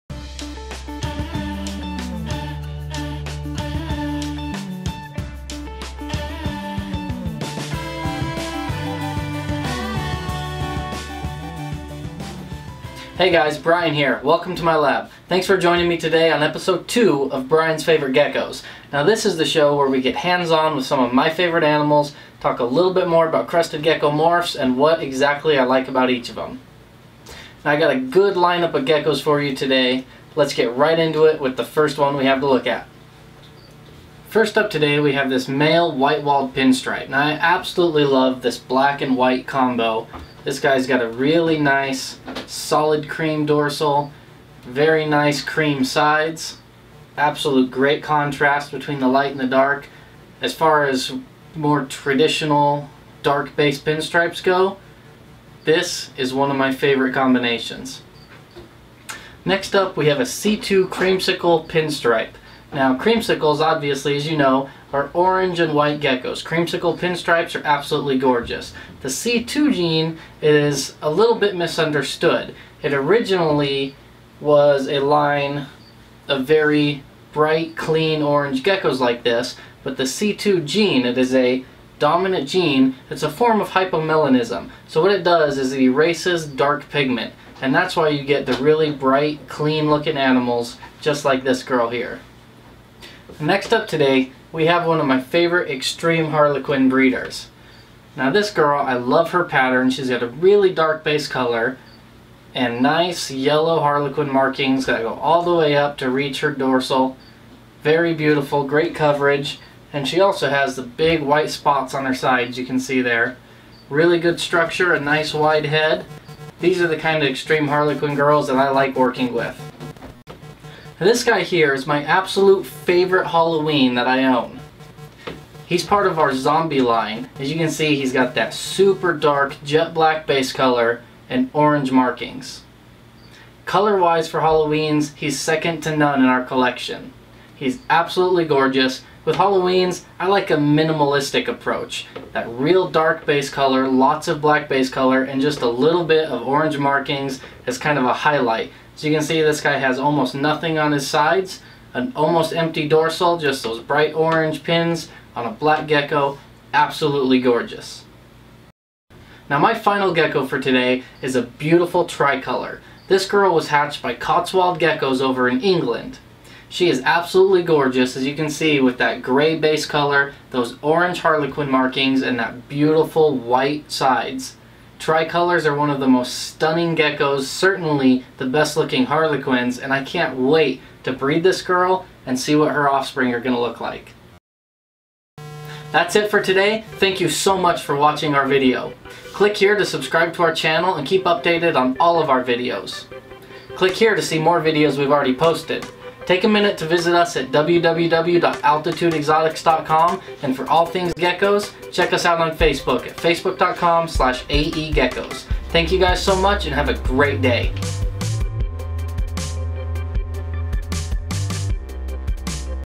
Brian's Favorite Geckos Episode 2 (White Wall, C2 Pinstripe, Extreme Harlequin and more!!)
Feel free to post any questions or suggestions of geckos you would like to see in a future episode.